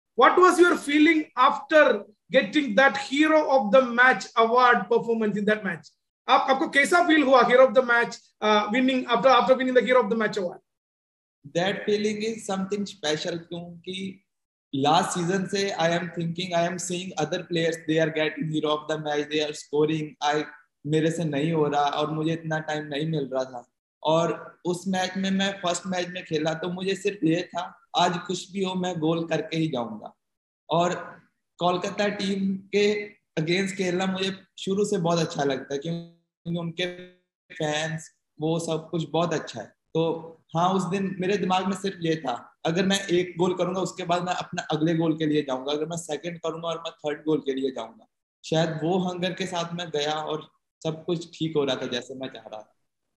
LFL Show - Vikram Partap Singh on winning HOTM award vs ATKMB
"That feeling was something special!"

Mumbai City FC's Vikram Partap Singh describes his emotions after winning the Hero of the Match award against ATK Mohun Bagan on the LetsFootballLive show! 💪